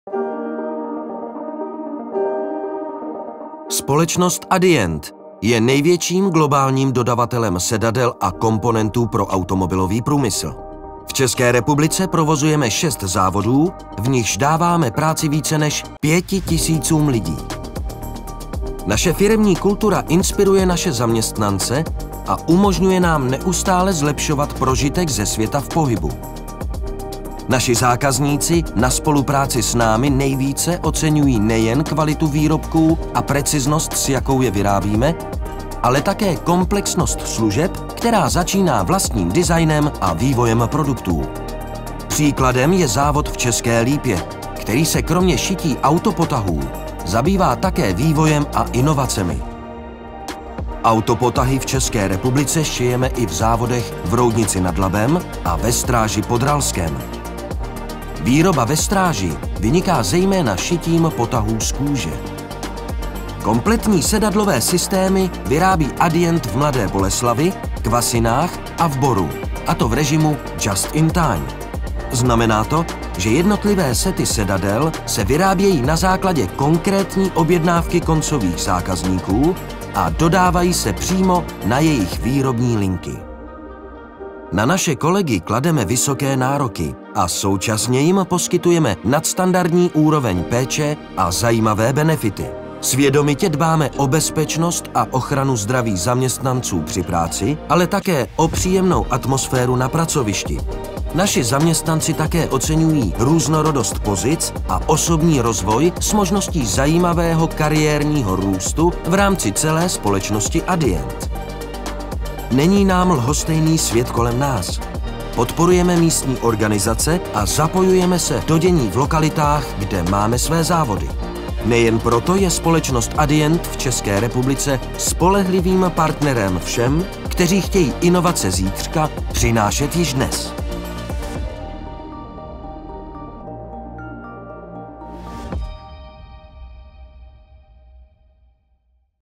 Adient ČR - HR prezentační video
Adient ČR vyrábí sedadla a komponenty pro automobily. Abychom kandidátům o práci představili české zastoupení společnosti Adient, vzniklo pro online prezentační a promo video. Během příprav natáčení jsme mysleli i na rozdíly, kterými se jednotlivé závody navzájem liší, a proto i jejich prezentace v rámci náboru má být v detailech odlišná. Natočili jsme dost materiálu pro to, aby mohly vzniknout další 3 varianty promo videa, které přesně komunikují informace dle potřeb jednotlivých závodů. Podívejte se, co každý z nich nabízí jiného a co mají společné.